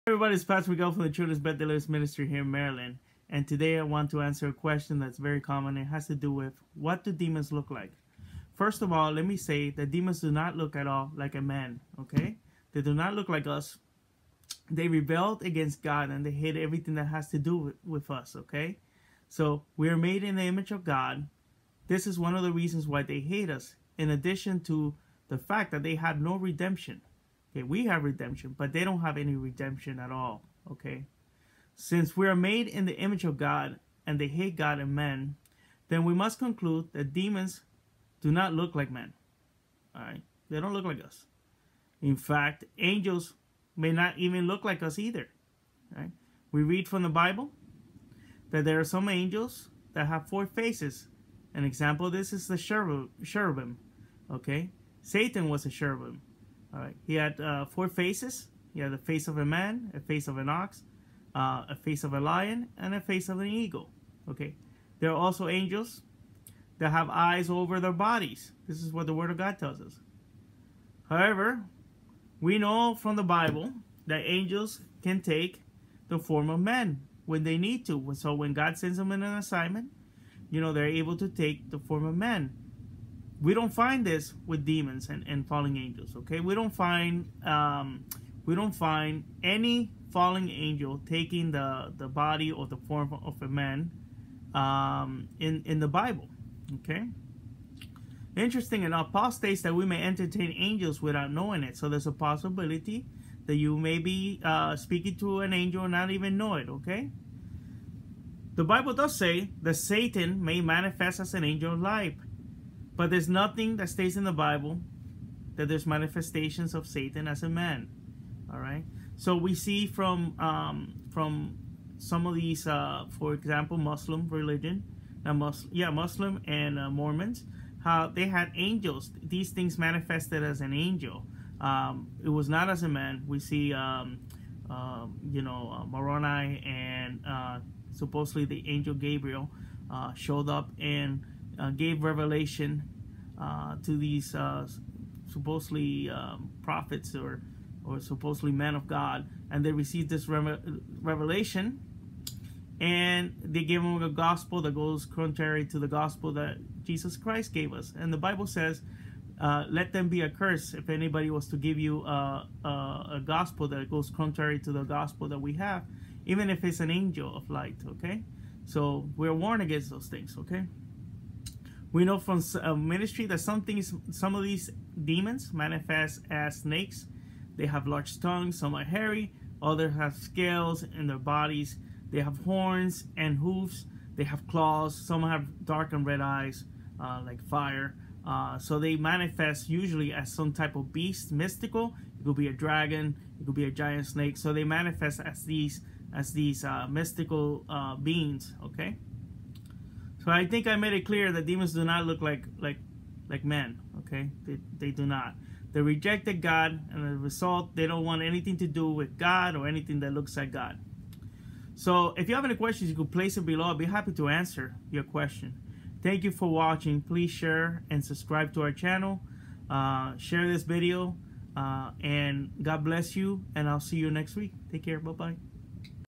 What do fallen angels and demons look like?
They do not look like men. Watch the video to find out. Miguel Bustillos and our team will help you get free in Jesus’ name!